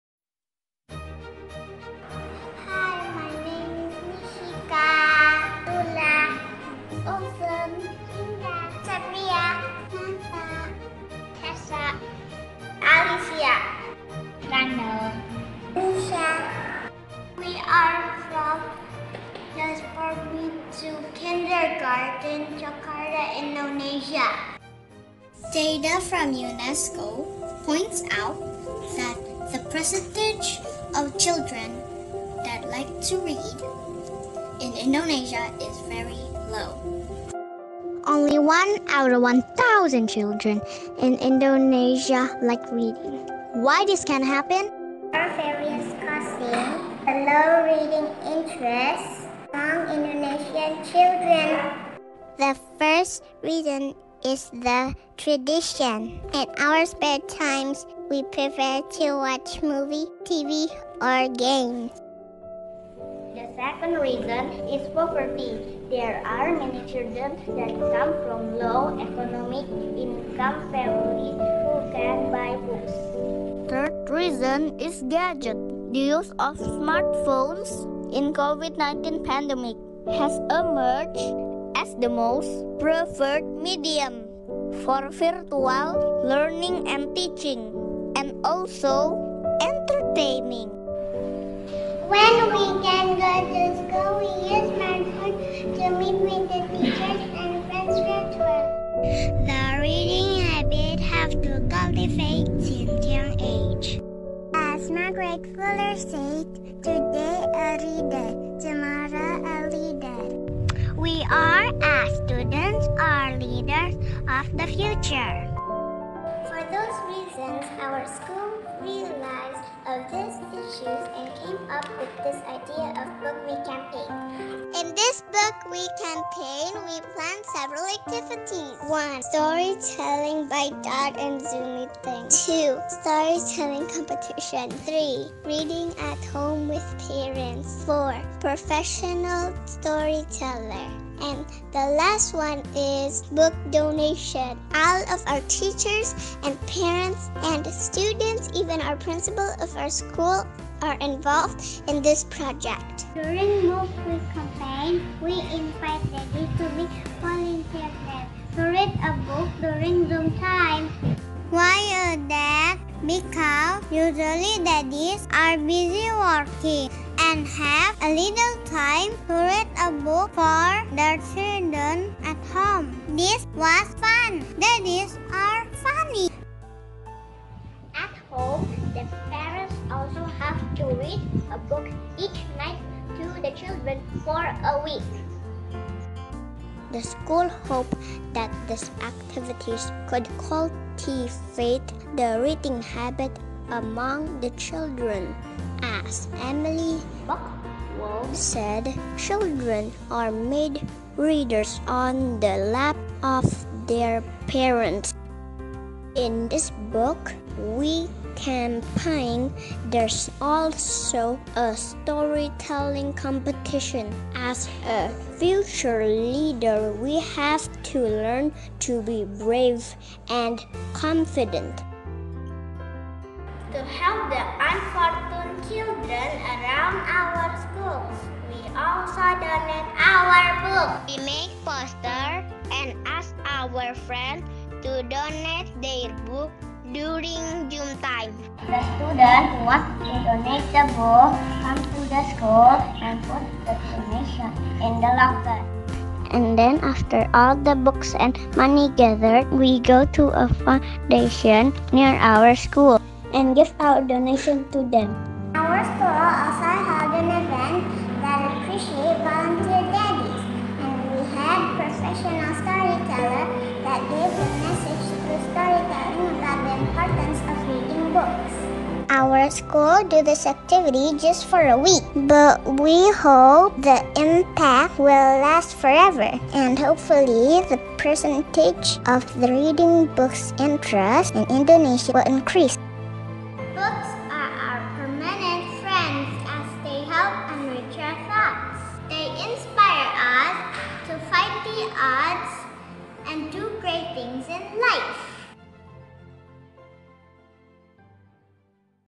Cultivate Reading Habits Through Book Week Campaign - Speech Contest LIM 2021 - TK Yasporbi 2
Tk Yasporbi 2 mengikuti lomba speech contest yang diadakan oleh Franklin Covey Foundation.